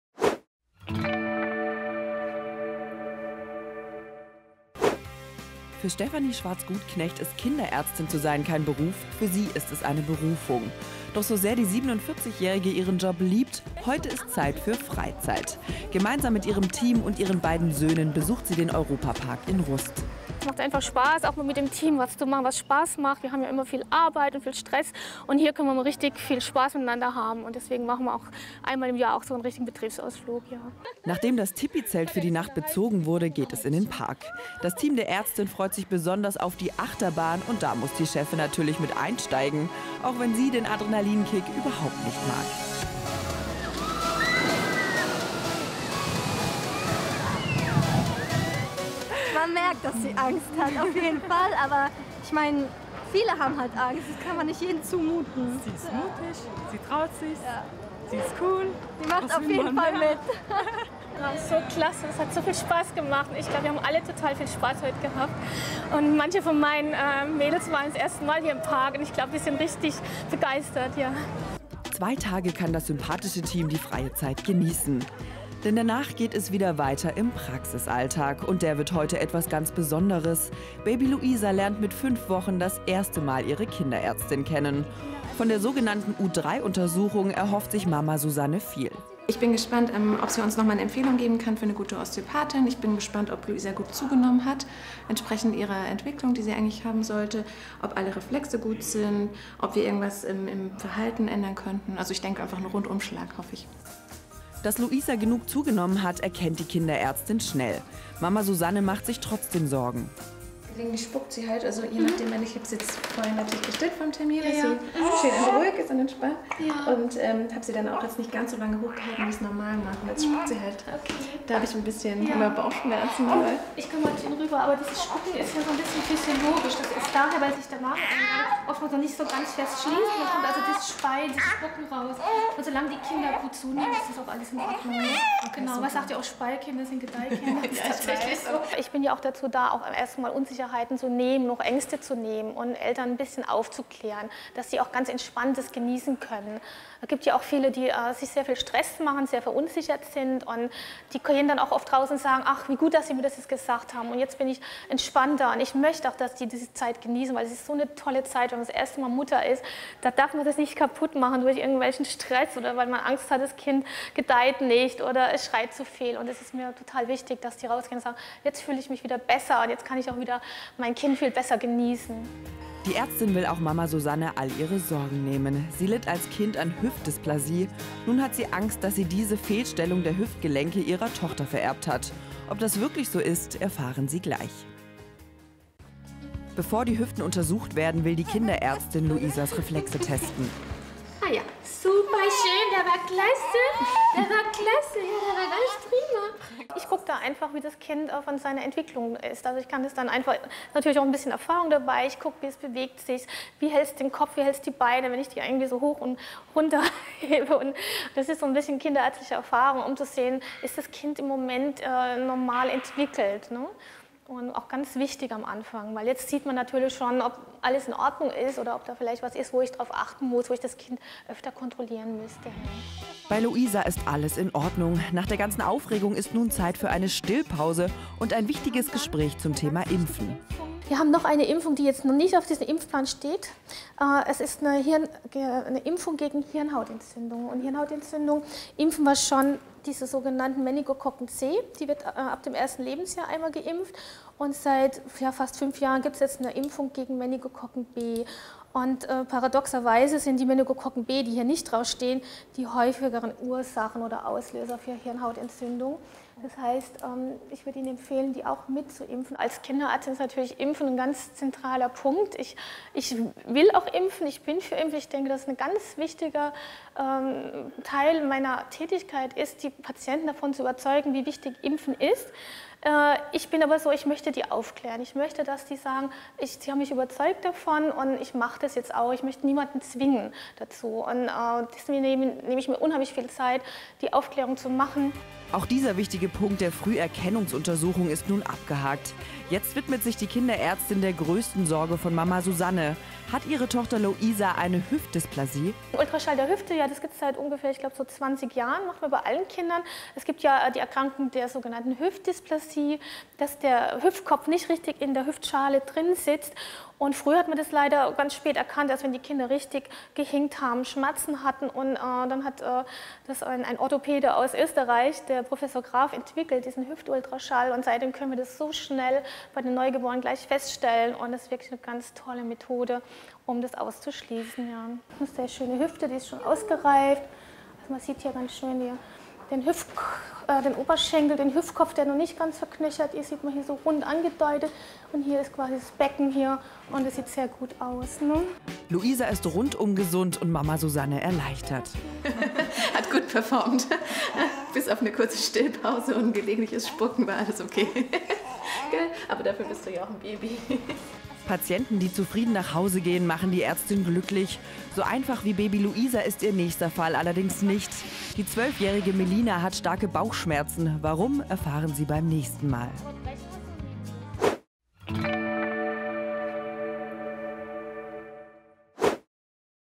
Begleitung einer Kinderärztin | RON TV
Heute geht es weiter mit unserer Wochenserie zur Mannheimer Kinderärztin Dr. Schwarz-Gutknecht. Wir waren dieses Mal bei ihrem jährlichen Betriebsausflug dabei und wir zeigen Ihnen eine ganz kleine Patientin, die ihren ersten aufregenden Besuch bei der Kinderärztin hat. Romy Schiemann hat dem Praxisteam dabei über die Schulter geschaut.